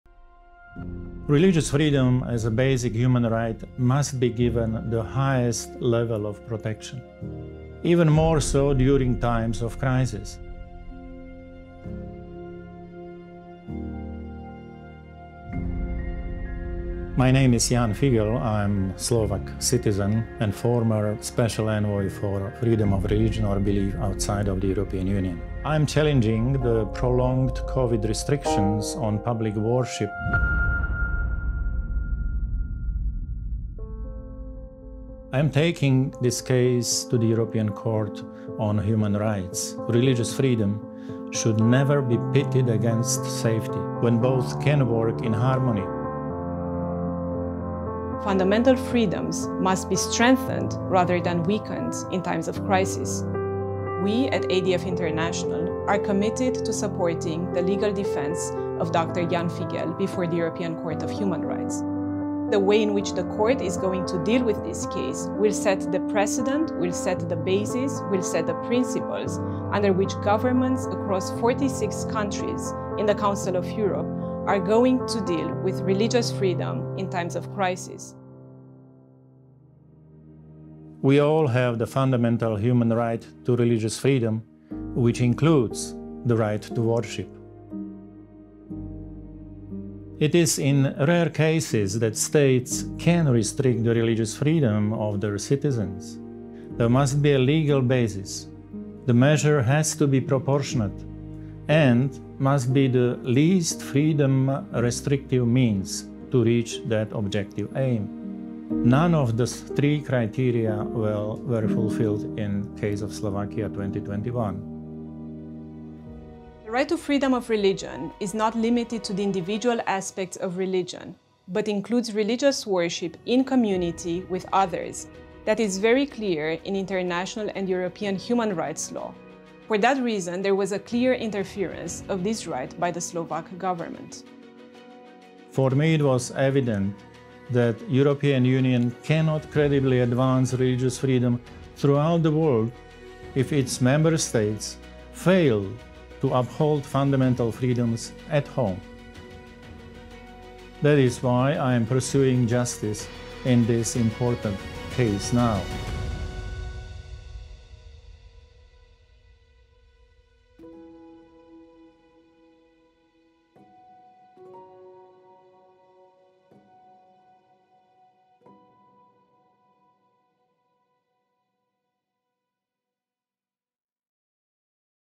Dr. Ján Figel’ - Defending Religious Freedom in Europe
The former Special Envoy for Freedom of Religion or Belief outside the EU, Dr. Ján Figeľ, has brought a challenge to the 2021 Covid restrictions on public worship in Slovakia at the European Court of Human Rights. The Court has officially communicated the case by formally notifying the Slovak government of the application and requiring a response. Thus, the case is one of the first ones where Europe’s top human rights court examines the impact of Covid restrictions on religious freedom in Europe.